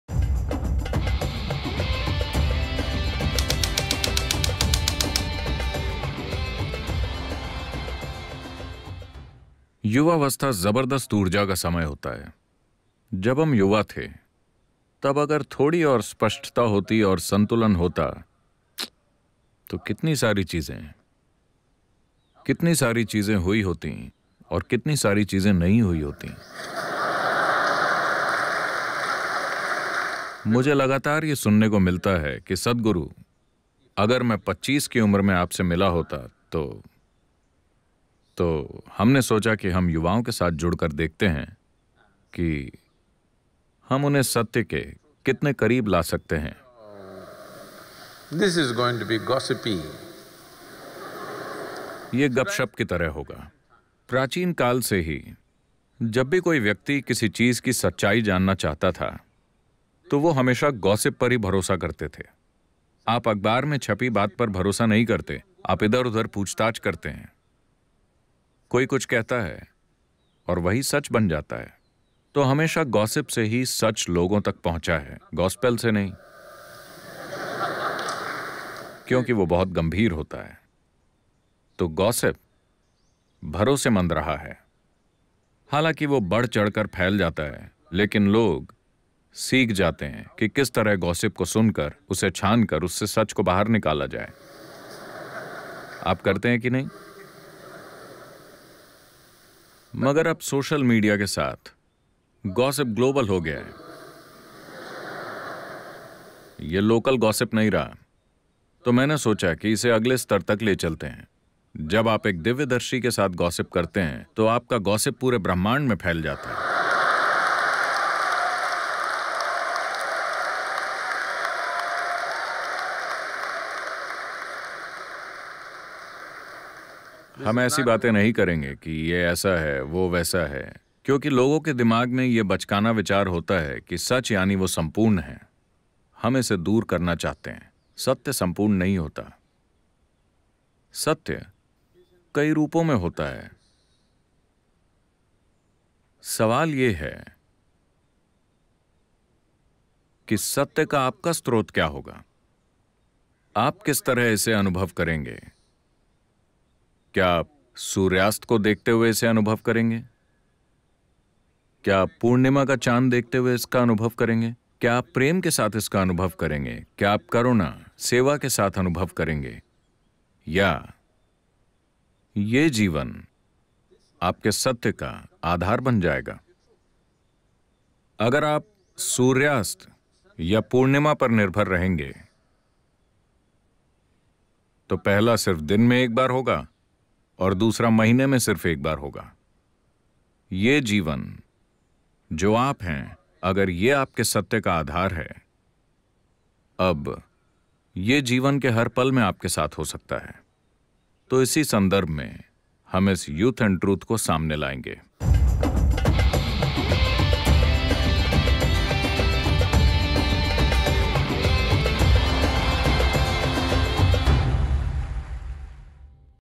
अब होगी सत्य पर गपशप! Ab hogi Satya par gapshap! in Hindi
सितंबर के महीने में सद्‌गुरु युवाओं को एक अनोखा मौका देने वाले हैं जहाँ युवा किसी भी विषय के बारे में अपने सवाल सद्‌गुरु से पूछ सकते हैं। Youth And Truth एक मौका होगा सत्य की खोज करने का, लेकिन उपदेशों की गंभीरता के साथ नहीं, बल्कि गपशप करते मजे में।

सद्‌गुरु से संवाद * अपना सवाल यहाँ पूछें: UnplugWithSadhguru.org